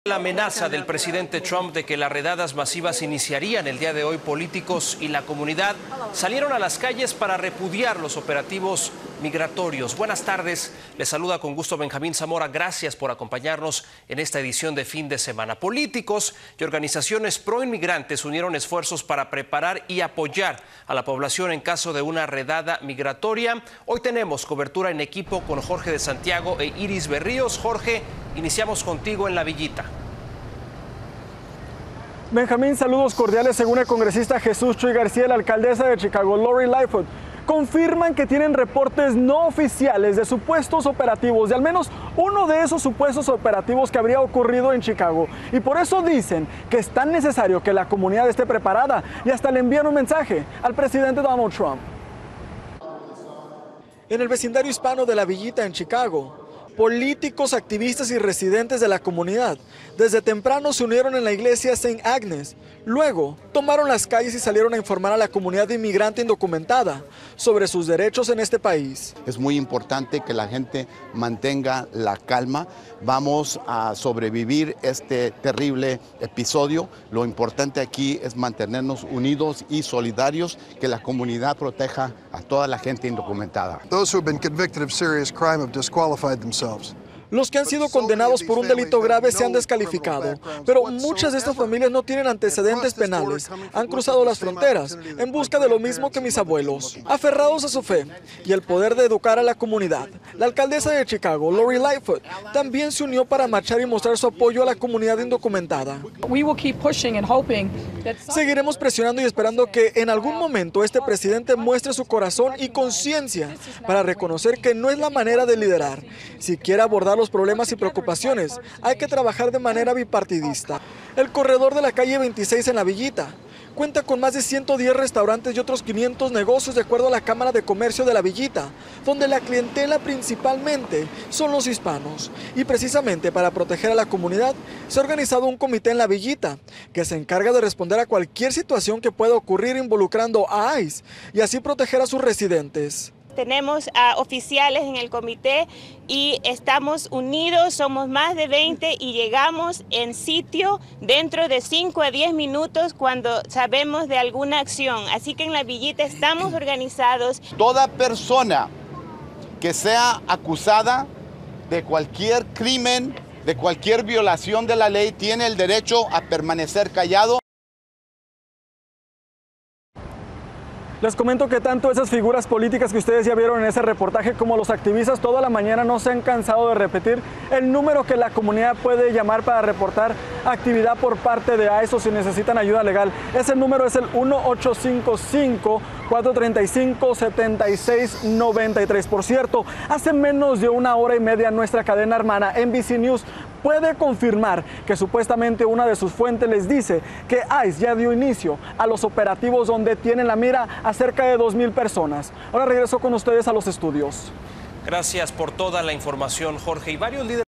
Chicago no se queda de brazos cruzados ante redadas de ICE | Telemundo Chicago
Políticos, activistas y religiosos se unen para proteger a las familias migrantes y afianzar su apoyo en medio de la tensión por el supuesto inicio de arrestos.